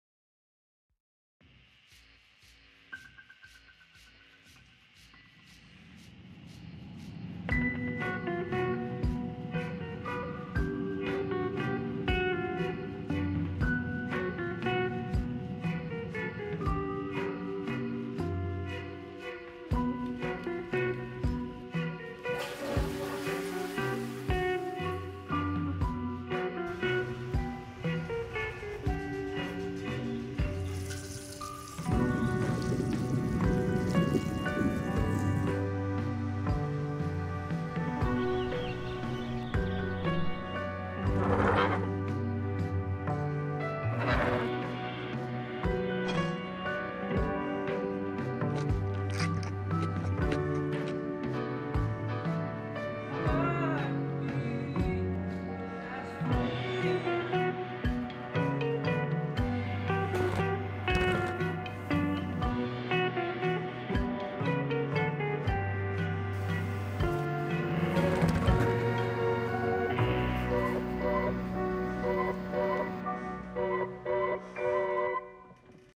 Mundane - Cinematic Short Film | Sony a6400
Experience the beauty in the "Mundane" with this cinematic montage of a typical day at home. From waking up to getting back to bed, the sequence is crafted in a lethargic, laid-back style. Whether you're looking for inspiration to find beauty in the ordinary or just want to sit back and enjoy a relaxing video, this cinematic montage has something for everyone. So grab some popcorn, sit back, and enjoy a unique perspective on a mundane day in my life.